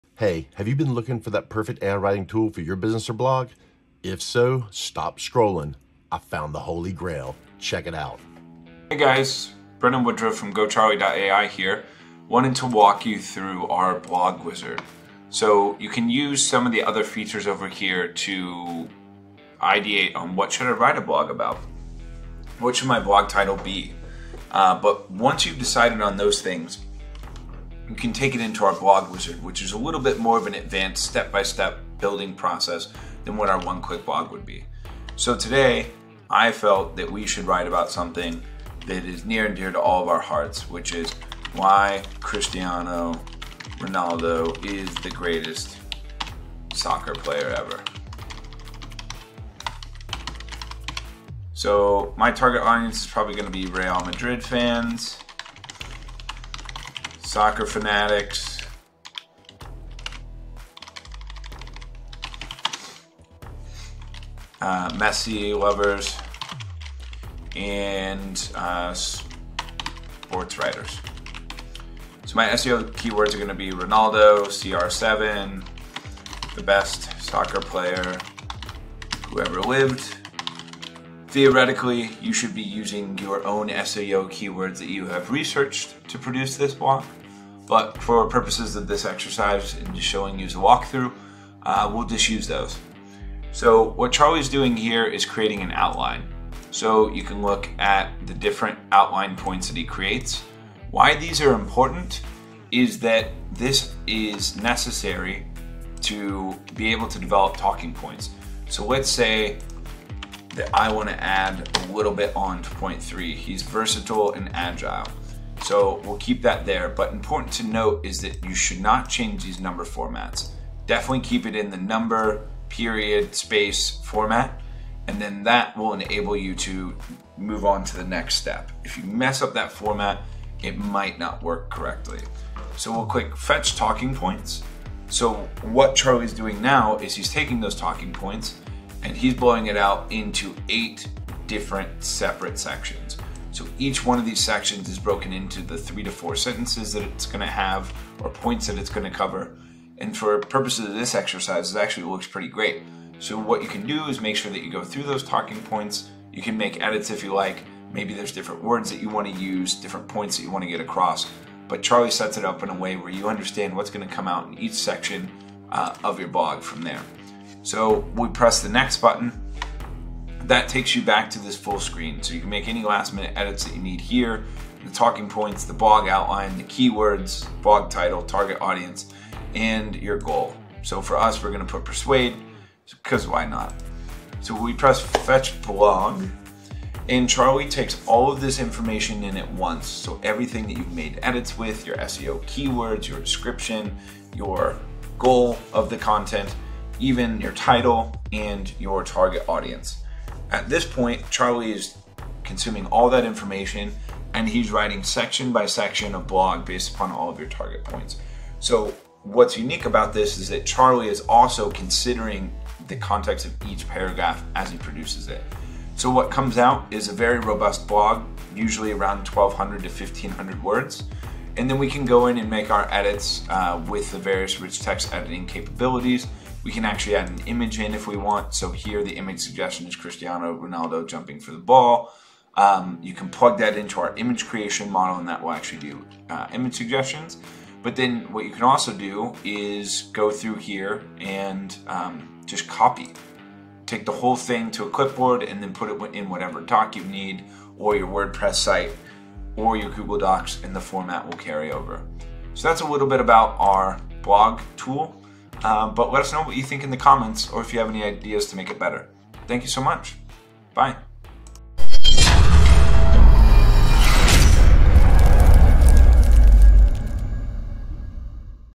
🤖 NEW AI Content Generator With FREE Trial: GOCharlie ai
GoCharlie ai is the ultimate content creator's best friend! With just one click or a few simple steps, you can generate a 1000+ word blog with Charlie's Blog Wizard. It also features a Text to Image tool, so you can easily turn your content into stunning visuals. GoCharlie ai makes it easy to create high-quality content that stands out from the crowd. With its intuitive user interface and powerful features, you can quickly create effective, engaging content that drives more readers to your blog. So don't settle for ordinary content - let GoCharlie ai help you create the content of your dreams!

✅ Today is the day it could all change.

Every business involves some risk. Please make sure to thoroughly research everything before making any decisions.